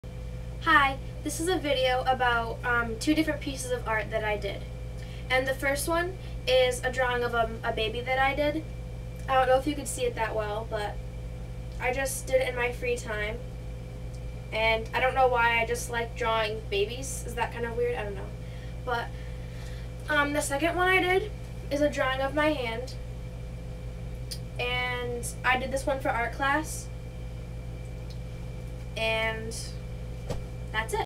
Two of My Drawings
I like drawing babies' faces. Is that weird?